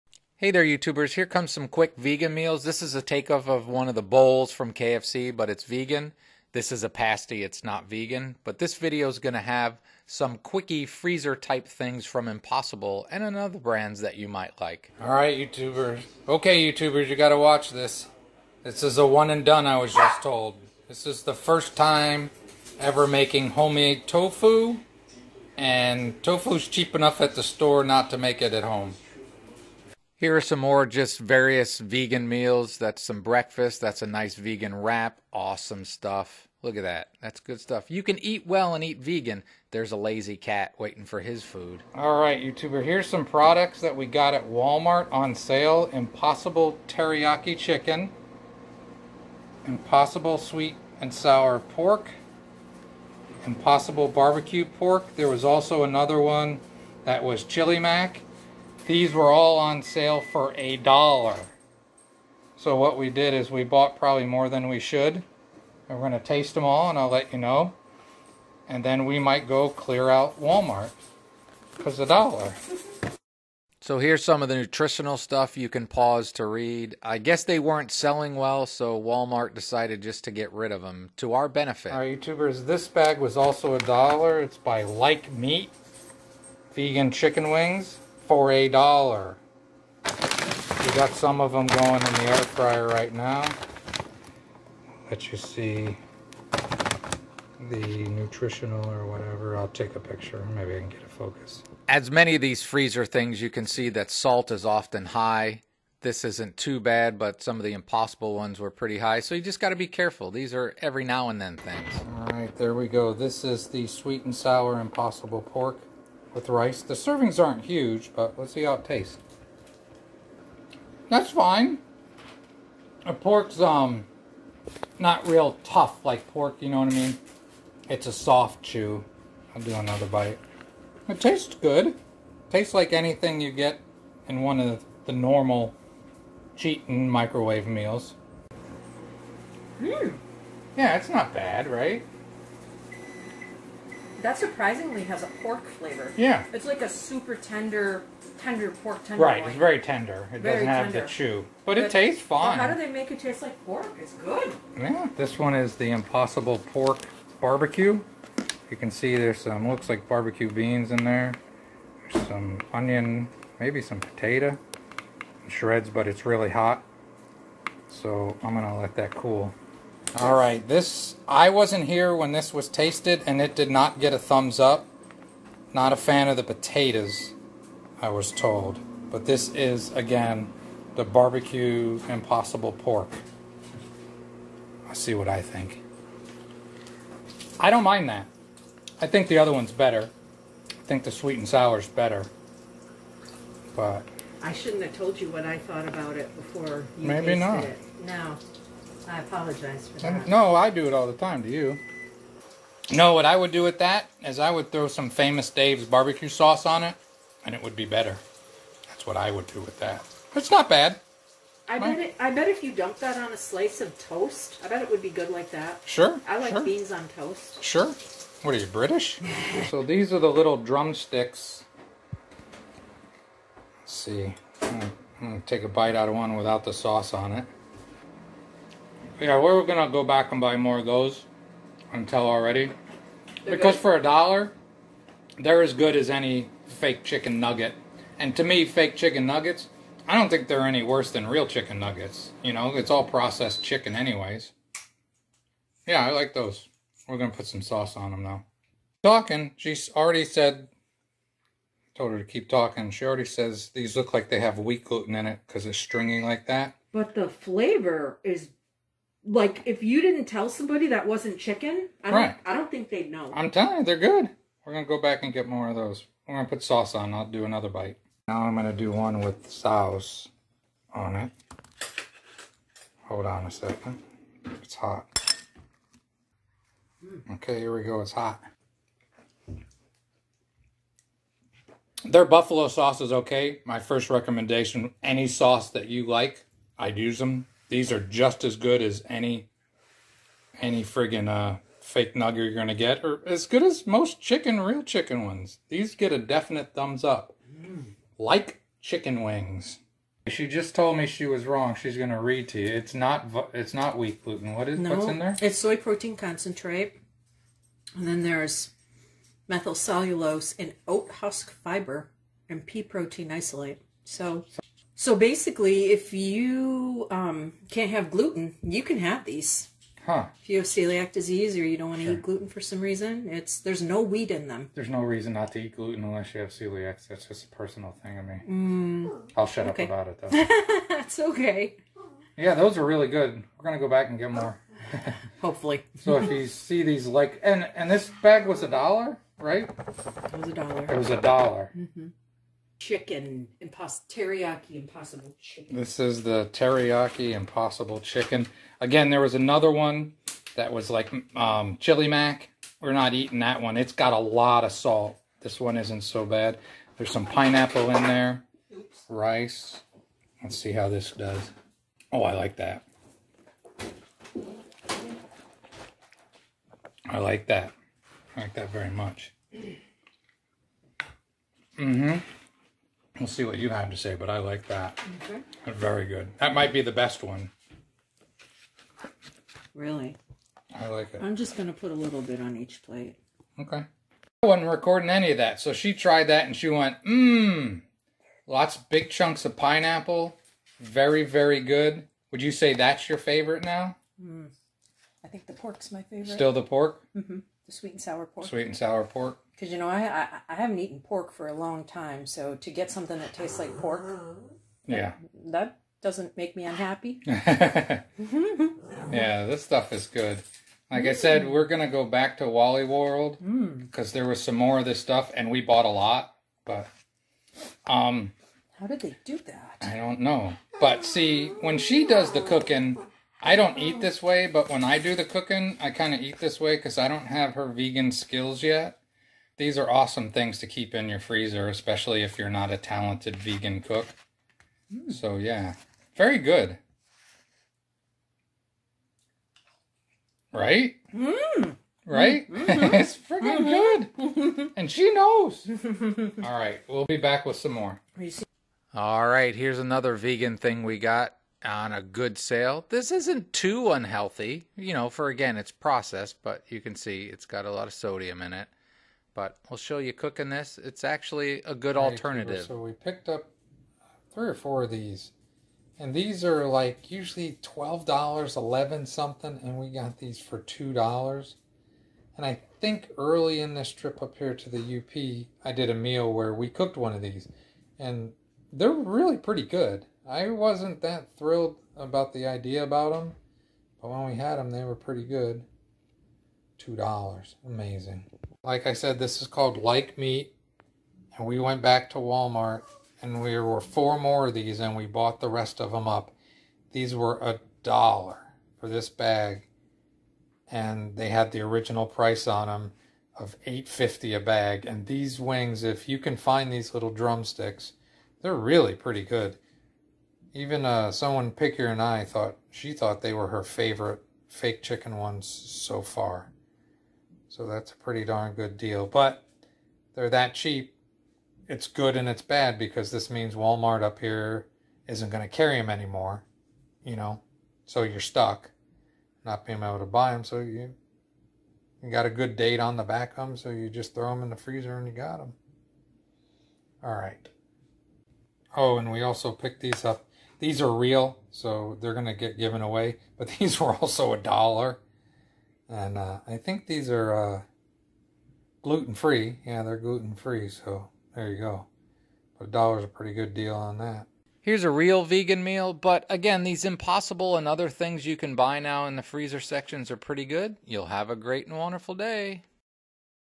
Vegan Quickie Meals ~ From the Freezer Section ~ Impossible Bowls (Amazing Deals)
Some vegan options (Impossible) from the grocery.